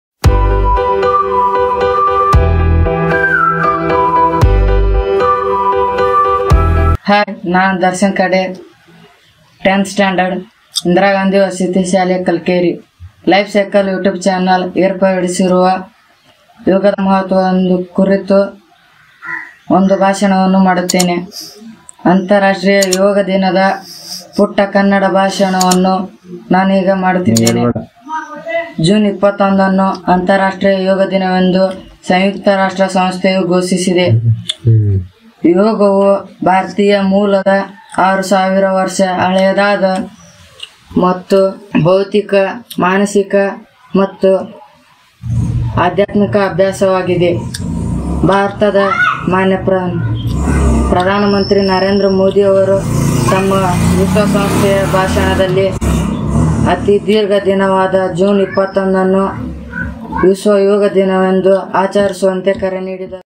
Lifecycle2021 International Yoga day speech in Kannada / Benefits / By Darshan karader
GEAR USED

* My camera
* My mic
* Tripod 1
* Tripod 2
* Memory card